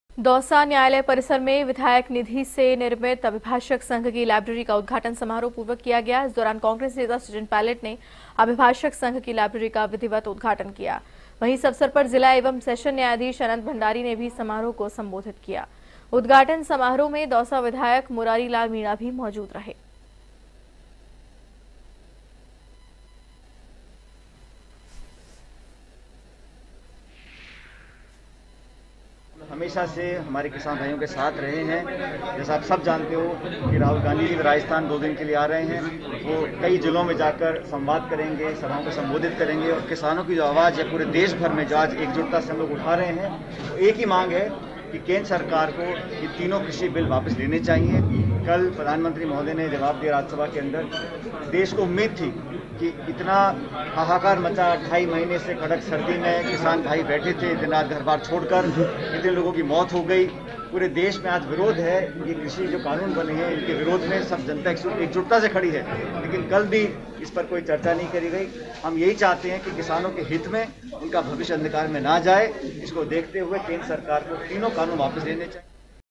Dausa: Rahul Gandhi के Rajasthan दौरे पर Sachin Pilot ने कही ये बात || Kisan Aandolan
A1TV  | Rajasthan News | Hindi News